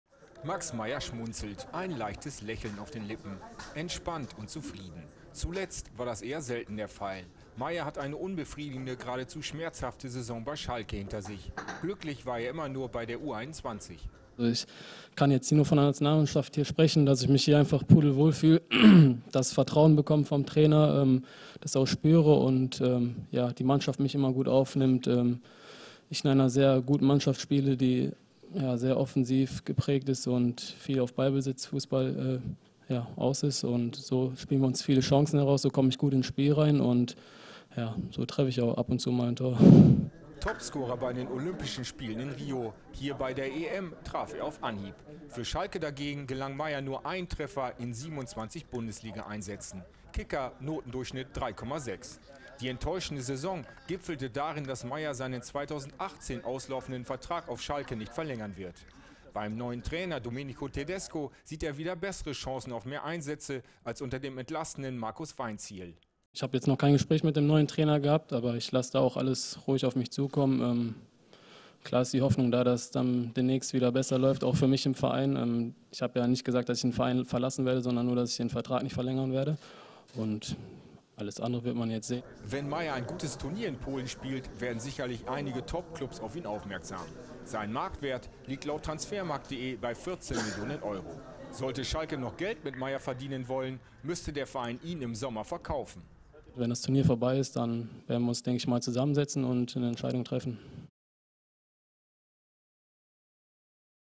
Meyer: "Habe ja nicht gesagt, dass ich Schalke verlasse"
Bei der U-21-EM in Polen ist Max Meyer derzeit mit der DFB-Auswahl gefordert, der Schalker zeigte sich beim Auftakt gegen Tschechien von seiner besten Seite. Nicht so in der vergangenen Spielzeit auf Schalke. Das "Warum" kann der 21-Jährige nicht beantworten. Seine Zukunftsplanung lässt Meyer, der seinen 2018 auslaufenden Vertrag bei S04 nicht verlängert hat, offen. Die Schlüsselfigur heißt Domenico Tedesco.